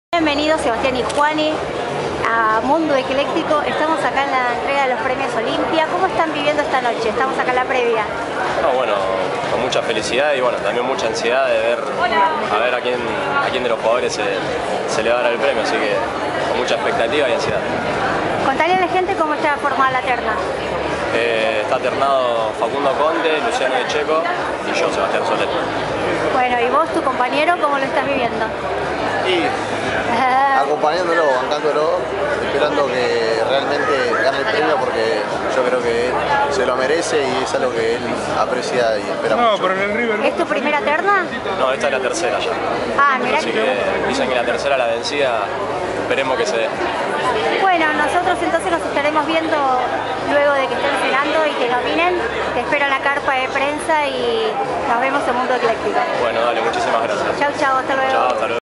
Mundo Ecléctico Cobertura: PREMIOS OLIMPIA 2012
Voleibol: Facundo Conte - Luciano De Cecco - Sebastián Solé

Los premios Olimpia 2012, se llevaron  a cabo el pasado martes 18 de diciembre de 2012  en el Salón Frers de La Rural, en la ciudad de Buenos Aires.
Todas las ternas correspondientes a 41 disciplinas
Ajedrez: Rubén Felgaer - Martín Lorenzini - Fernando Peralta
Atletismo: Juan Manuel Cano - Germán Lauro - Braian Toledo
Automovilismo: Mauro Giallombardo - Marcos Ligato - José María López
Básquetbol exterior: Carlos Delfino - Emanuel Ginóbili - Luis Scola
Básquetbol local: Facundo Campazzo - Juan Gutiérrez - Leonardo Gutiérrez
Billar: Ariel Casto - Walter Ingegnieri - Juan Pablo Sisterna
Bochas: Raúl Basualdo - Guillermo Montemerlo - Cristian Zapata
Boxeo: Sergio Martínez - Lucas Matthysse - Omar Narváez
Canotaje: Miguel Correa/Rubén Rezola Voizard - Daniel Dal Bo - Alexandra Keresztesi/Sabrina Ameguino
Cestoball: María Cecilia Lábaque - María Eugenia Lescano - Dana Mariani
Ciclismo: Walter Pérez - Maximiliano Richeze - Sebastián Trillini
Equitación: Martín Dopazo - Alejandro Madorno - Leandro Moschini
Esgrima: José Domínguez - Alberto Pérez Ghersi - María Belén Pérez Maurice
Esquí náutico: Lorena Botana - Javier Julio - Jorge Renosto
Fútbol exterior: Sergio Agüero - Gonzalo Higuaín - Lionel Messi
Fútbol local: Sebastián Domínguez - Lisandro López - Ignacio Scocco
Fútbol Sala: Santiago Basile - Santiago Elías - Luis González
Gimnasia: Milagros Carrasco Pini - Federico Molinari - Valeria Pereyra
Golf: Ariel Cañete - Emiliano Grillo - Andrés Romero
Handball: Andrés Kogovsek - Diego Simonet - Sebastián Simonet
Hockey sobre césped: Luciana Aymar - Florencia Habif - Rocío Sánchez Moccia
Judo: Emanuel Lucenti - Paula Pareto - Cristian Schmidt
Karate: Franco Icasati - Giuliana Novak - Julián Pinzás
Lucha: Patricia Bermúdez - Yuri Maier - Luz Vázquez
Motociclismo: Ezequiel Iturrioz - Alejandro Patronelli - Marco Solorza
Natación: Damián Blaum - Pilar Geijó - Sofía Sánchez/Etel Sánchez
Padel: Fernando Belasteguin - Cristian Gutiérrez - Cecilia Reiter
Patín: Luis De Mattía - Mariángeles Mantuano - Victoria Rodríguez López
Pato: Andrés Lanfranco - Ariel Tapia - Nicolás Taverna
Pelota: Javier Nicosía - Alfredo Villegas - Gabriel Villegas
Pesas: Emerson Burla - Iván Palacios - Joana Palacios
Polo: Pablo Mac Donough - Gonzalo Pieres (H) - Juan Nero
Remo: María Laura Abalo/Gabriela Best - Santiago Fernández - Cristian Rosso/Ariel Suárez
Rugby: Patricio Albacete - Juan Martín Fernández Lobbe - Gonzalo Gutiérrez Taboada
Softbol: Francisco Lombardo - Bruno Motroni - Carla Villalva
Taekwondo: Sebastián Crismanich - Carola López - Martín Sio
Tenis: Carlos Berlocq - Juan Martín Del Potro - Juan Mónaco
Tenis de Mesa: Ana Codina - Liu Song - Pablo Tabachnik
Tiro: Fernando Gazzotti - Melisa Gil - Alex Suligoy
Turf: Altair Domingos - Gonzalo Hahn - Jorge Ricardo
Yachting: Julio Alsogaray - Lucas Calabrese/Juan De La Fuente - Mariano Reutemann
Paralímpico: Hernán Barreto - David Effrón - Jorge Lencina